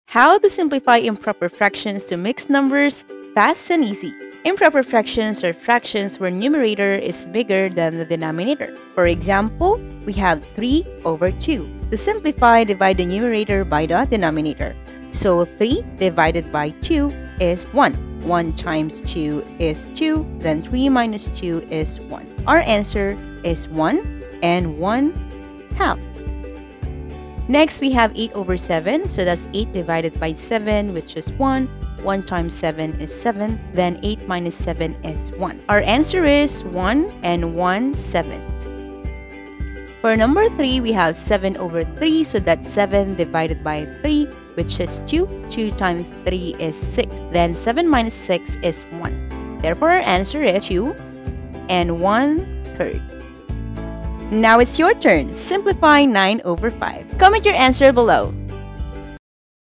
How to Simplify Improper Fractions to Mixed Number
Need to simplify improper fractions? Here are the easy steps for you!

Be part of the family!